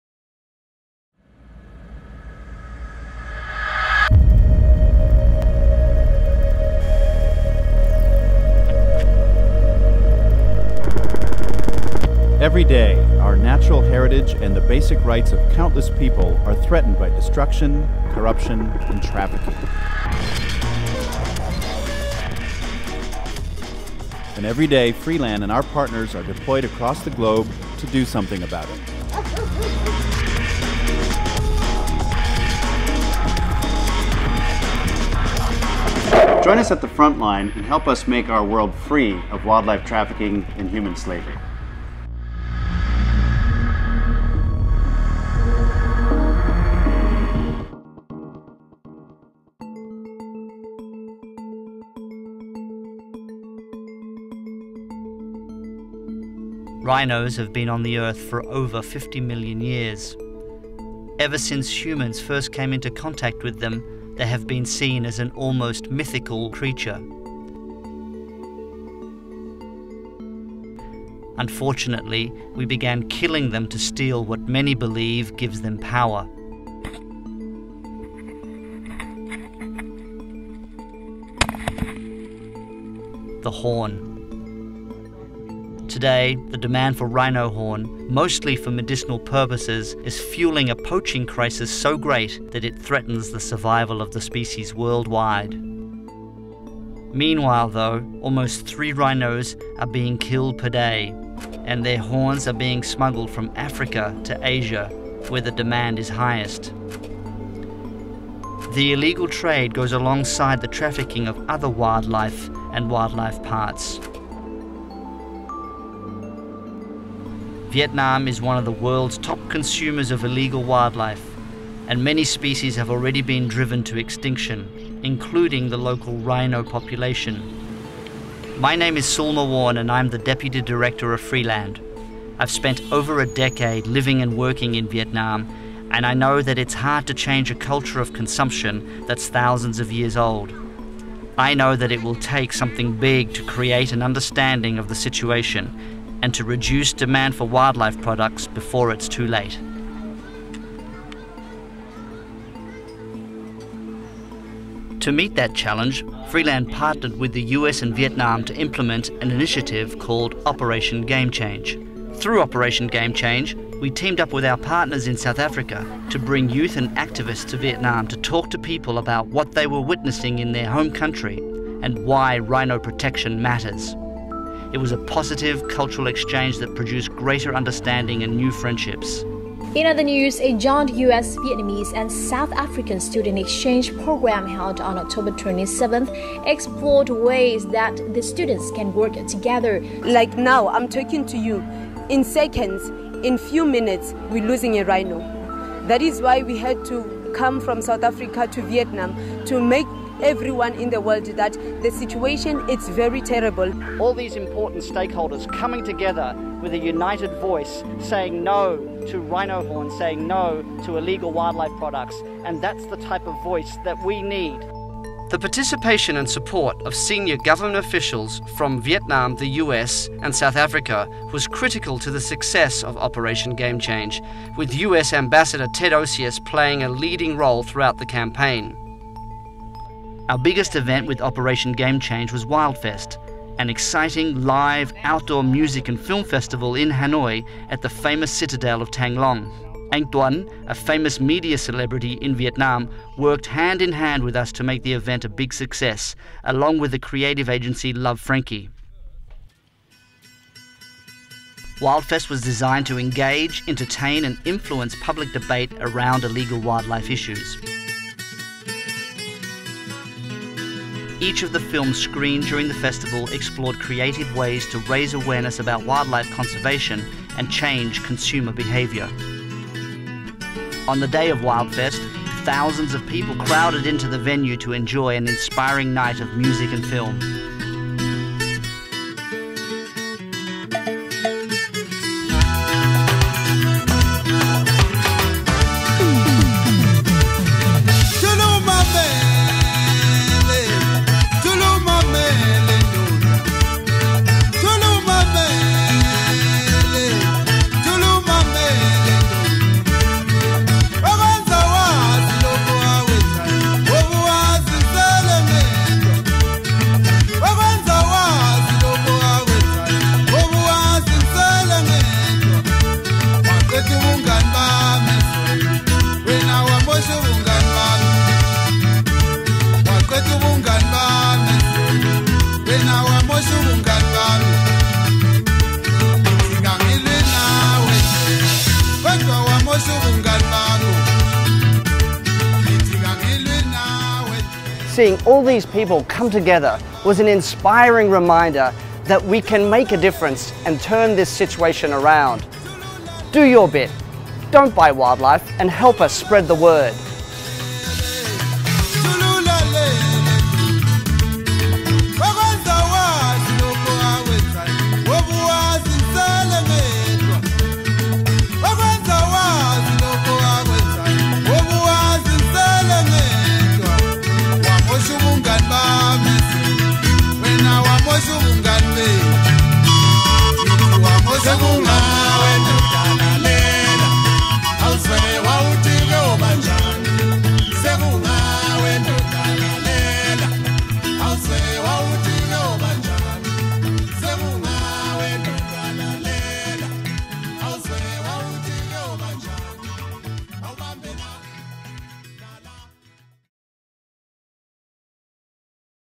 WildFest
Vietnam and the United States teamed up to positively influence millions of Vietnamese by raising public awareness of wildlife crime and the consumption of endangered species, especially rhino horn.